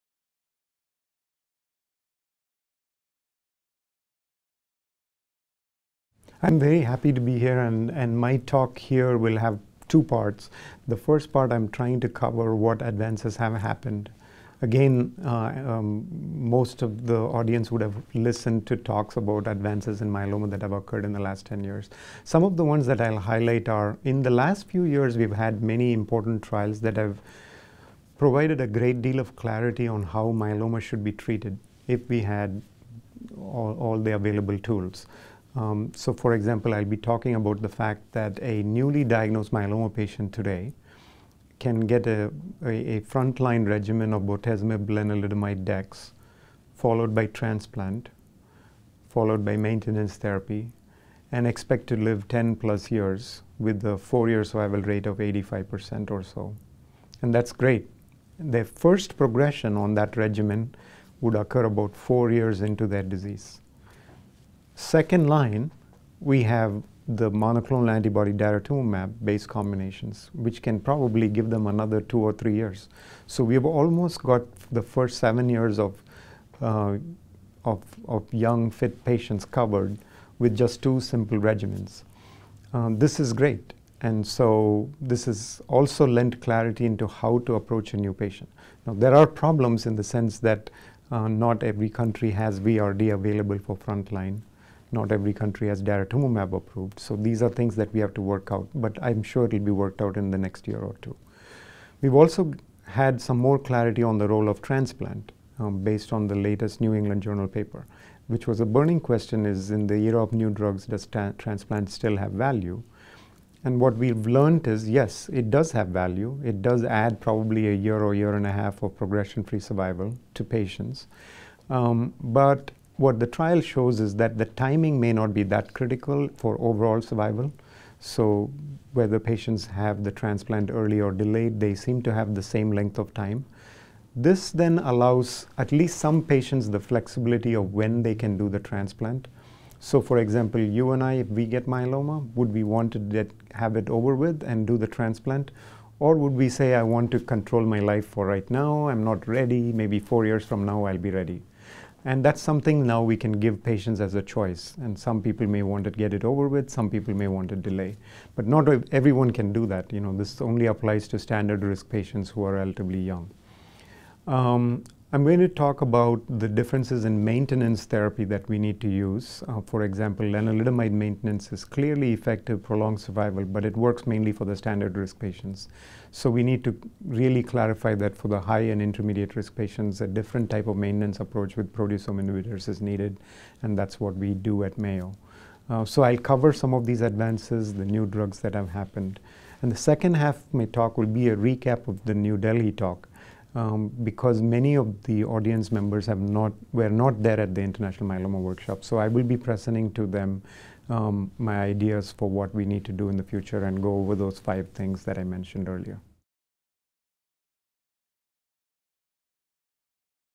Advances in multiple myeloma treatment over the last years
Vincent Rajkumar, MD, from the Mayo Clinic, Rochester, MN, discusses the advances of the last few years in multiple myeloma (MM) treatment at the 2017 World Congress on Controversies in Multiple Myeloma (COMy) in Paris, France. Dr Rajkumar explains that recent clinical trials have shed light on the best way to approach to treating myeloma patients. He argues that a newly diagnosed patient, who receives a frontline regimen of VRD (bortezomib-lenalidomide-dexamethasone), followed by and in combination with transplantation and subsequent maintenance therapy can expect to live ten or more years. For second-line, the combinations based on the monoclonal antibody daratumumab can further improve the patient’s life expectancy.
In addition, Dr Rajkumar speaks about the value of transplantation in this age of new therapeutic agents and how recent studies determined that the timing may not be particular important for overall survival (OS), allowing some patients flexibility on when to perform the transplant. He concludes discussing the differences in maintenance therapy and its effectiveness between different subsets of patients.